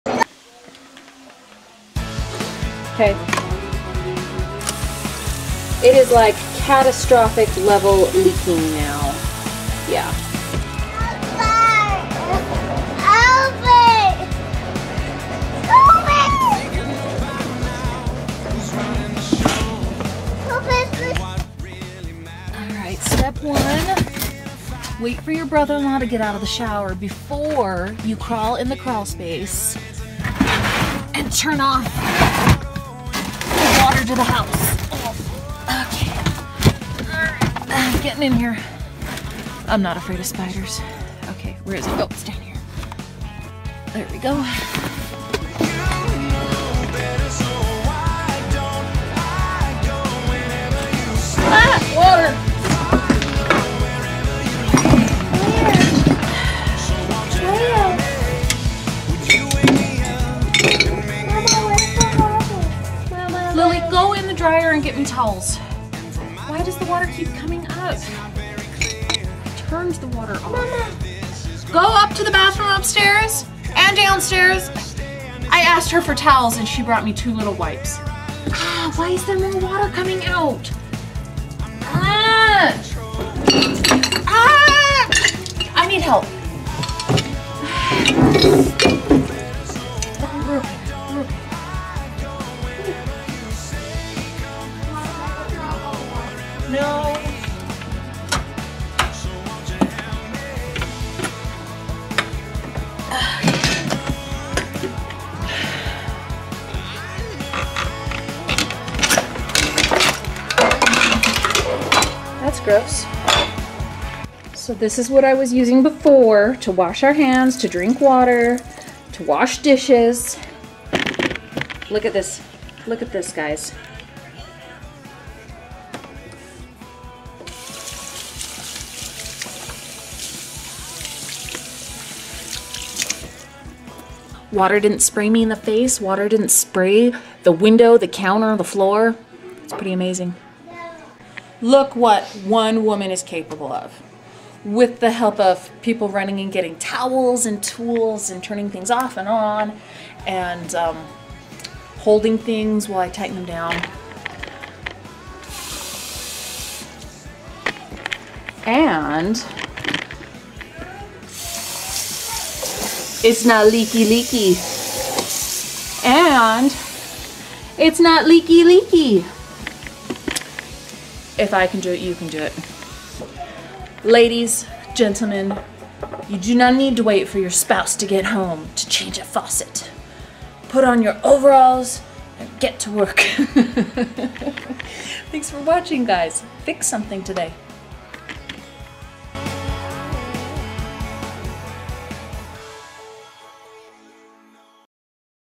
Don't Wait, Replace
Ladies and Gentlemen, don't wait for your man or woman to get home to replace that leaky faucet.  Put on your overalls and get to work.

Music, Mercy Me